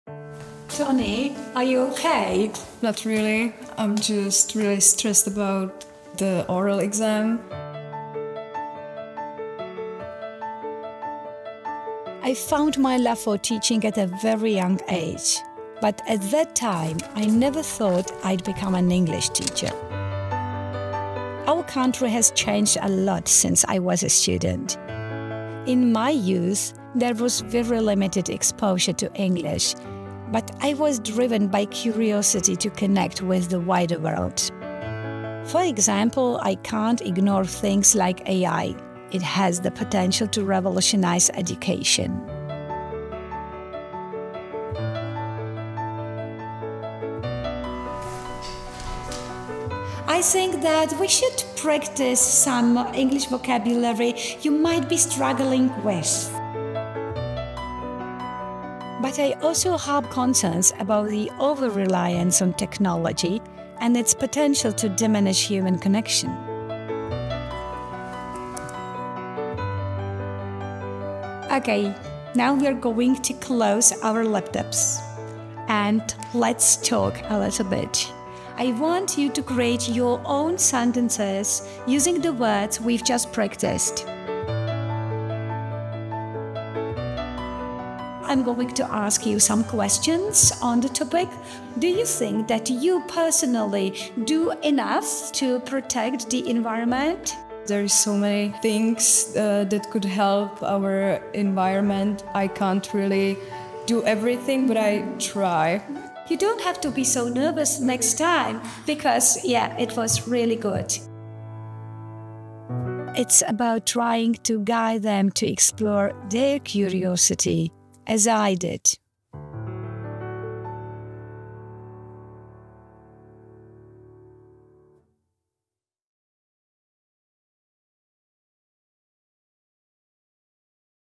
Zuzana - Discover the joys of teaching
“I was driven by curiosity to connect to the wider world."​

Every lesson we give our best, despite the challenges and sometimes slow progress. We celebrate together with our students those small moments of satisfaction when the participate, of pride when they share, and of joy when they achieve.

Discover the joys of teaching.

With thanks Zuzana Kočová and students at Krkonošské gymnázium a Střední odborná škola, Czech Republic.